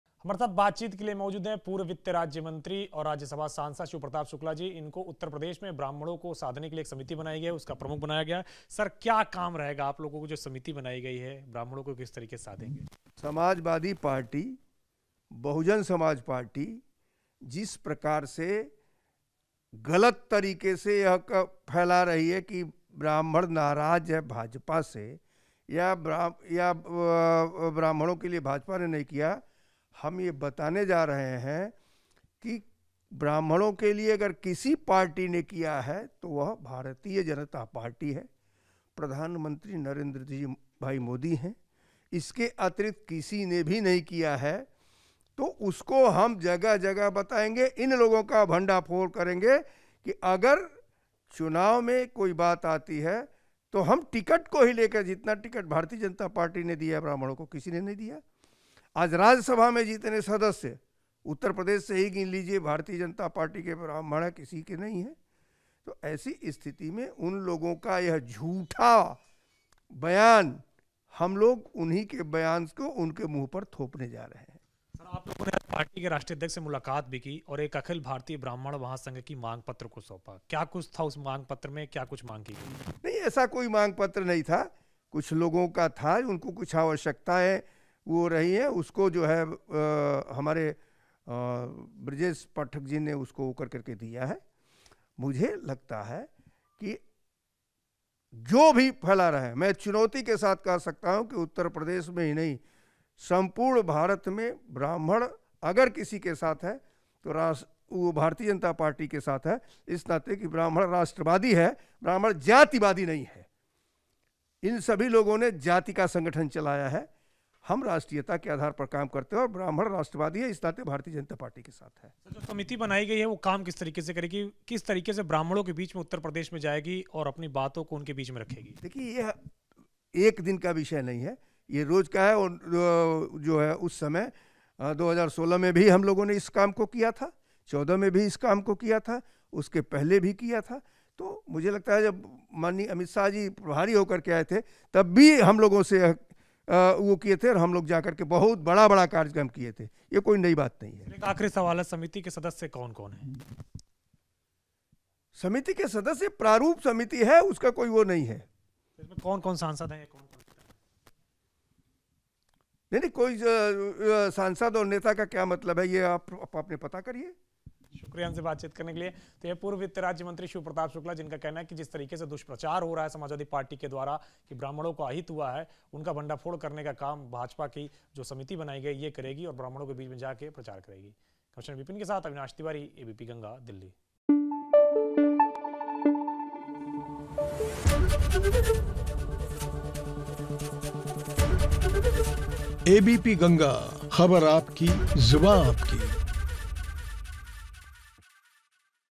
UP Election: यूपी में ब्राह्मणों पर BJP की नजर, समिति अध्यक्ष शिव प्रताप शुक्ला ने क्या जानकारी दी ?
यूपी चुनाव पास आते ही सभी पार्टियां एक्टिव मोड में आ गई हैं। यूपी में अब ब्राह्मण समाज को साधने की कोशिश की जा रही है। जिसके लिए एक समिति बनाई गई है।  जिसका अध्यक्ष
शिव प्रताप शुक्ला को बनाया गया है। उनसे इस मामले पर खास बातचीत हुई...देखिए क्या बोले..